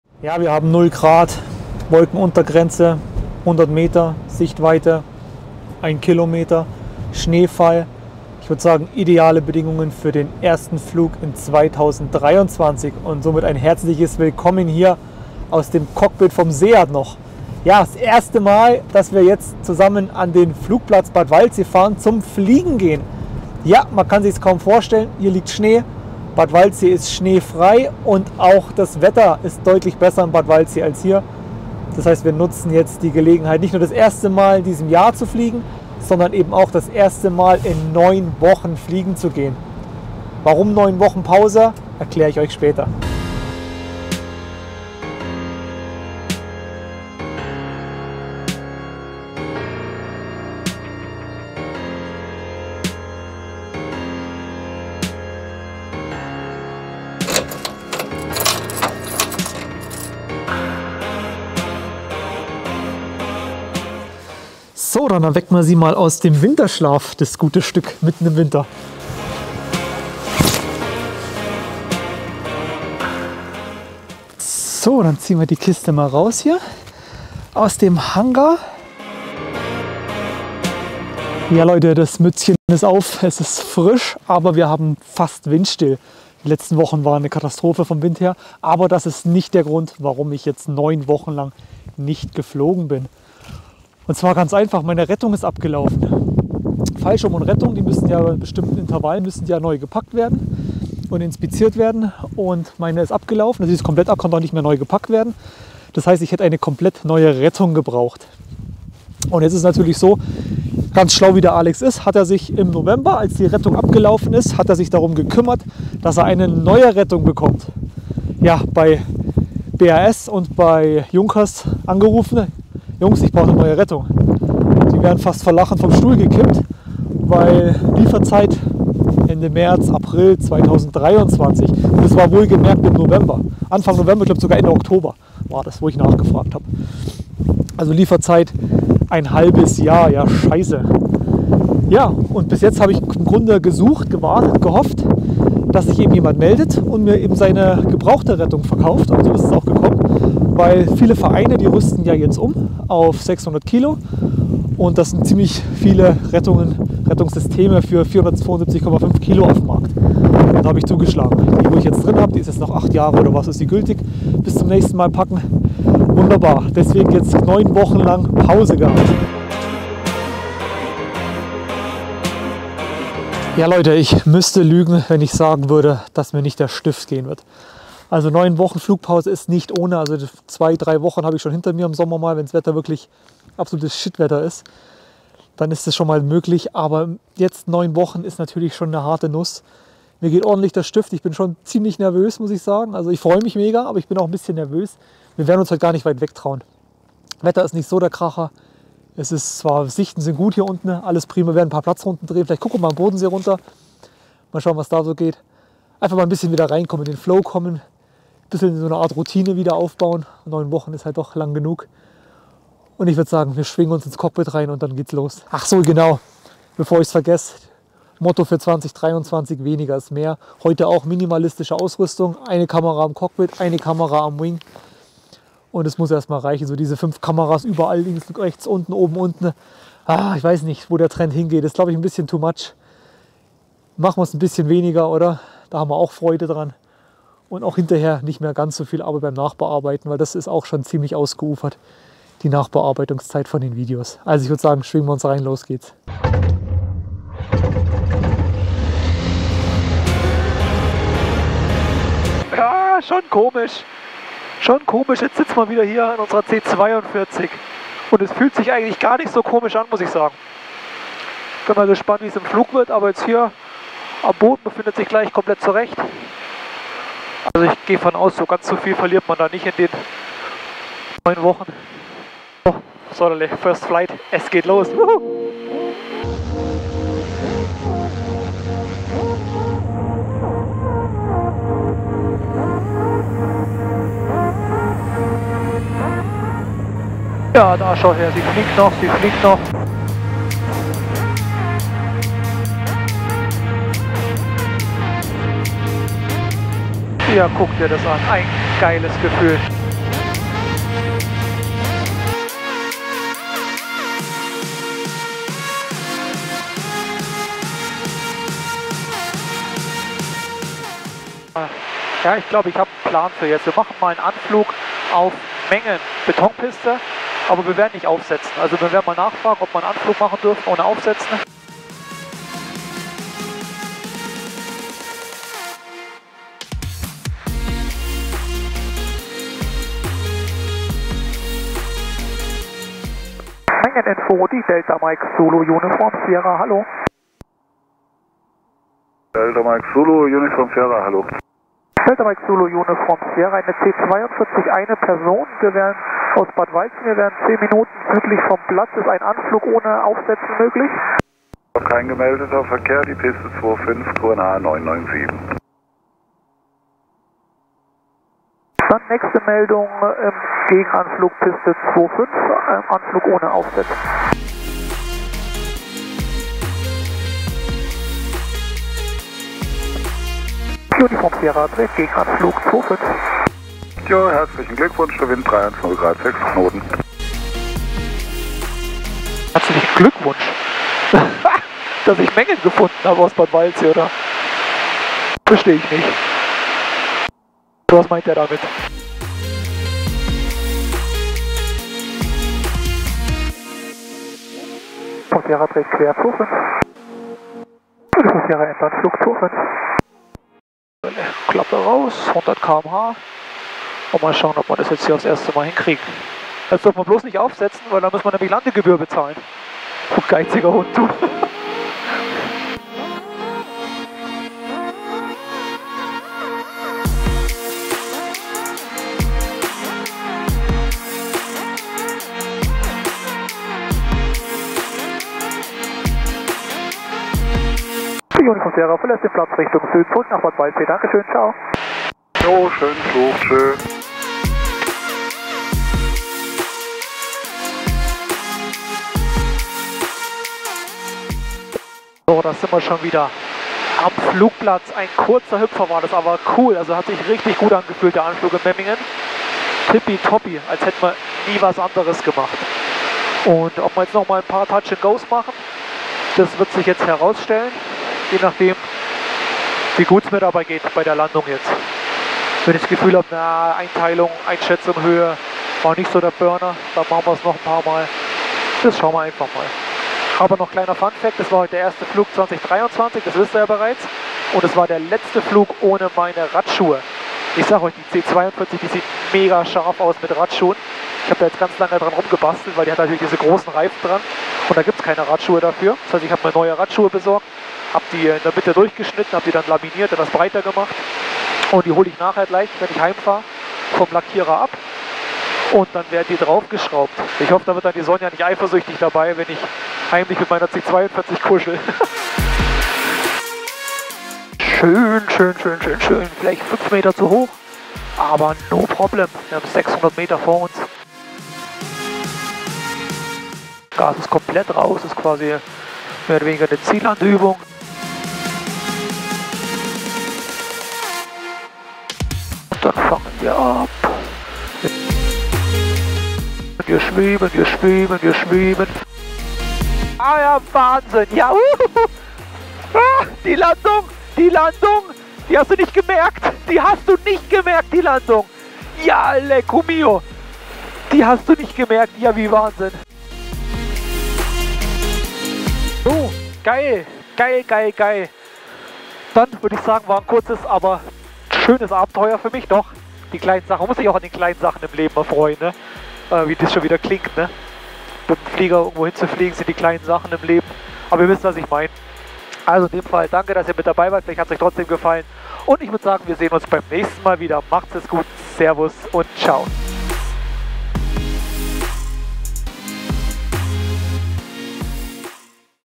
Erstflug im Ultraleichtflugzeug nach 9 Wochen Pause. Überflug Mengen. Comco Ikarus C42
Nach 9 Wochen Zwangspause war es jetzt endlich wieder so weit. Der Erstflug in 2023 stand an und auch der erste Flug seit über 9 Wochen. Warum und wie es mir dabei ergangen ist erfahrt ihr in diesem Video.